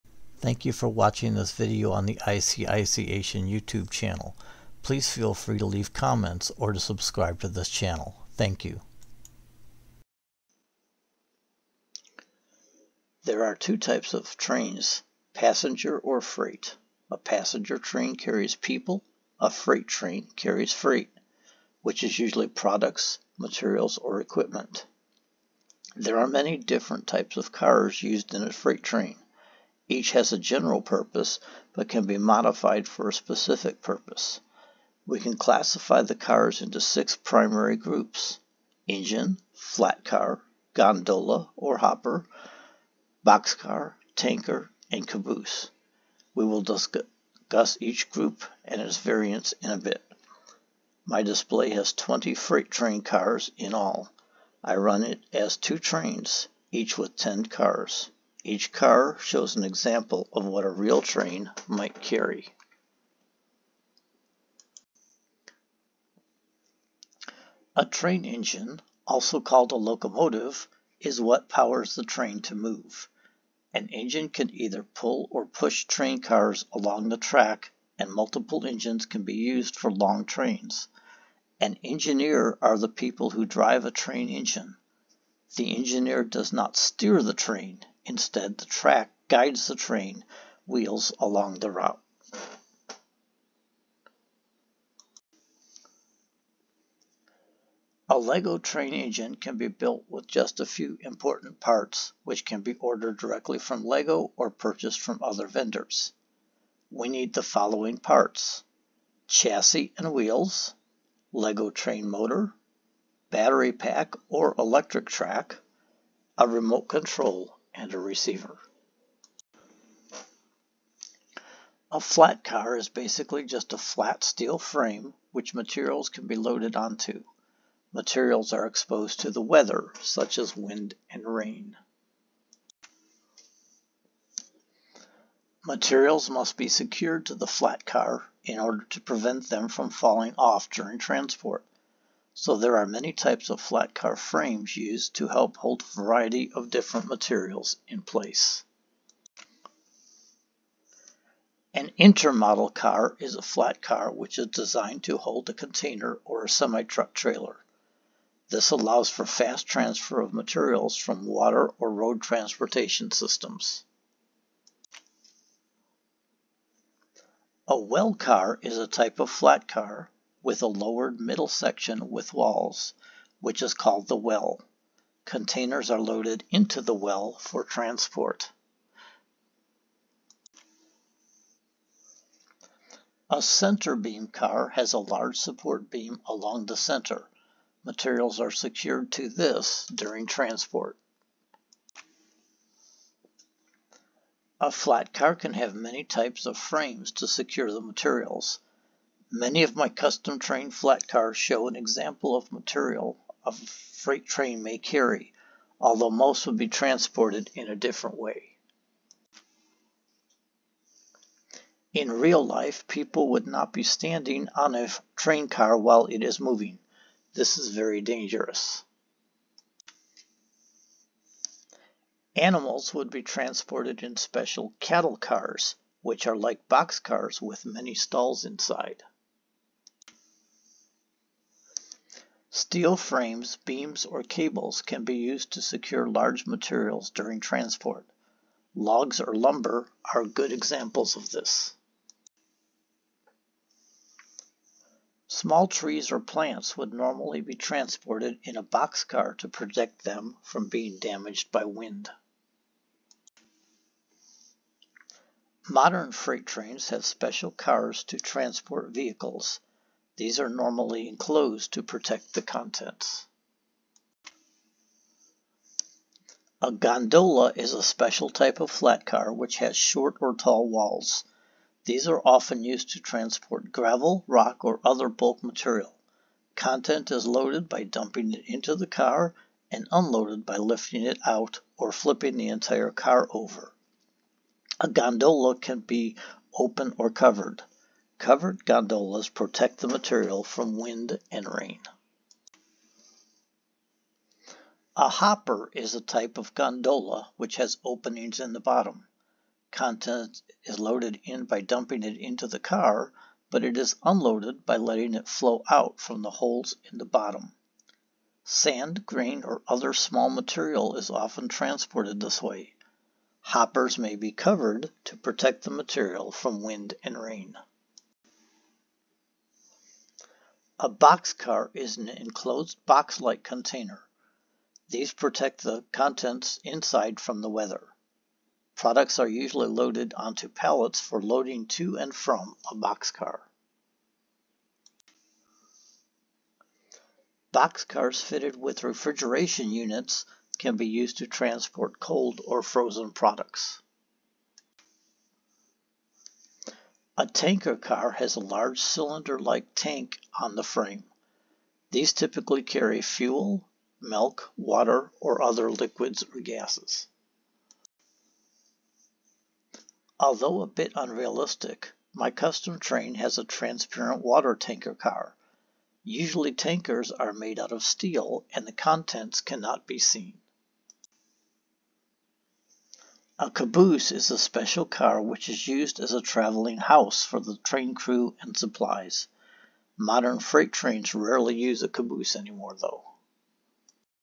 Custom Lego train
This is a video I created to go along with my custom Lego train display. It provides some information on trains in general as well as showing each of the 20 train cars I have created so far.